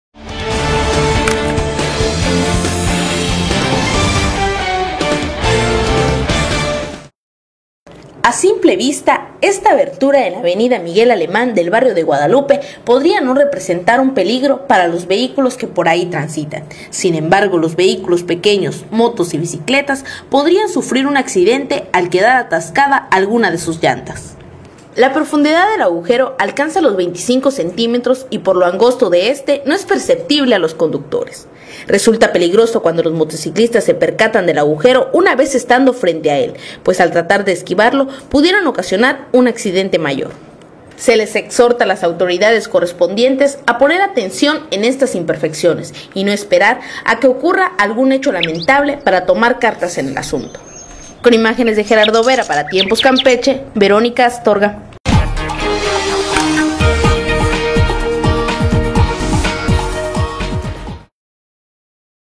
Peligroso agujero en la Av. Miguel Aleman
A simple vista esta abertura en la Av. Miguel Alemán del Barrio de Guadalupe podría no representar un peligro para los vehículos que por ahí transitan, sin embargo los vehículos pequeños, motos y bicicletas podrían sufrir un accidente al quedar atascada algunas de sus llantas.

La profundidad del agujero alcanza los 20 centímetros y por lo angosto de éste no es perceptible a los conductores.

Resulta peligroso cuando los motociclistas se percatan del agujera una vez estando frente a él, pues al tratar de esquivarlo pudieran ocasionar un accidente mayor.

Se les exhorta a las autoridades correspondientes a poner atención en estas imperfecciones y no esperar a que ocurra algún hecho lamentable para tomar cartas en el asunto.